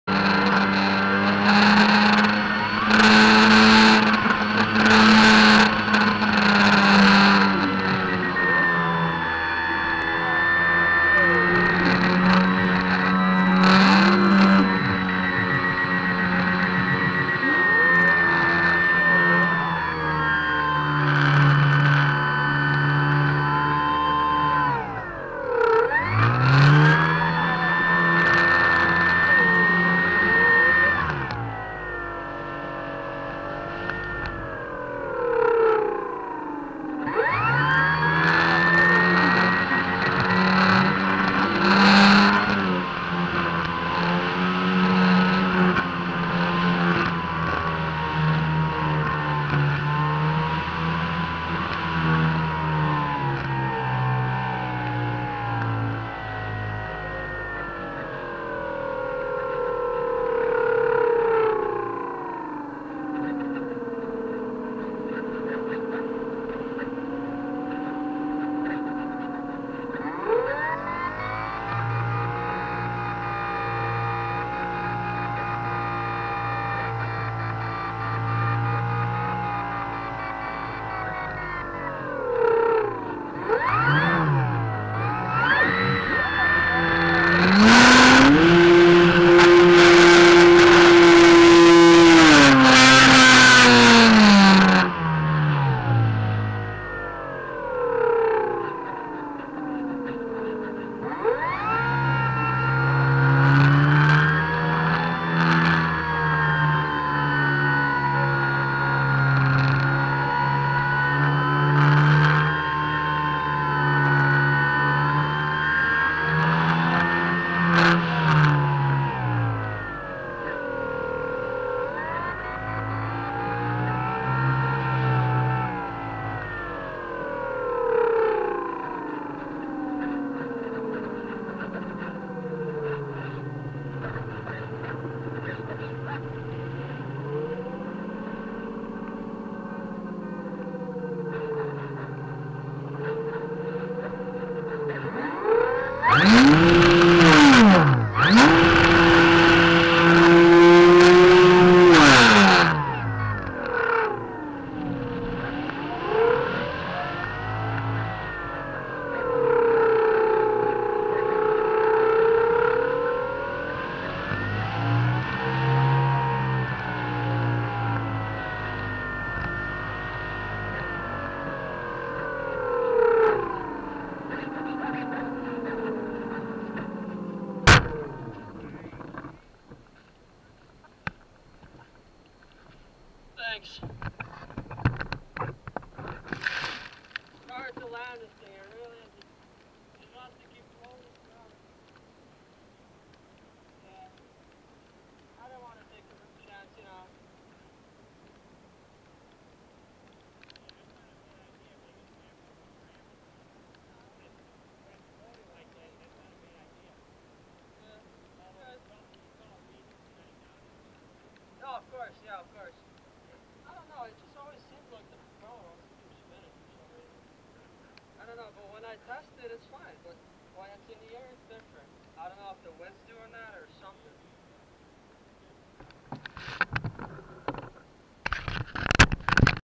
Mystique 2.9M RES flight 3 part 3 of 3
in this video i test out how the E-flite Mystique 2.9M RES handles in wind speeds between 15-20 MPH and gusting to 26-30. this is at the Raven Glen Forest Preserve in Lake County, IL.